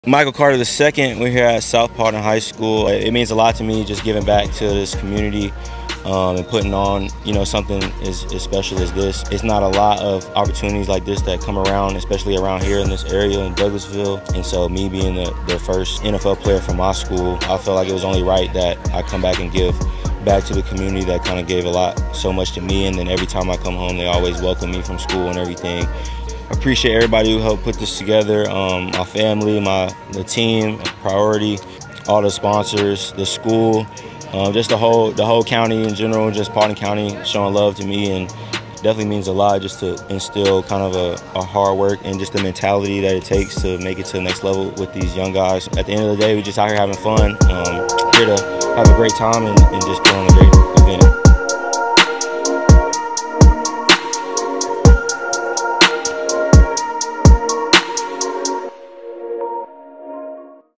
2023 MC2 Camp Recap
Michael Carter II of the NY Jets hosted his first annual youth football camp at his home high school, South Paulding in Douglasville, GA!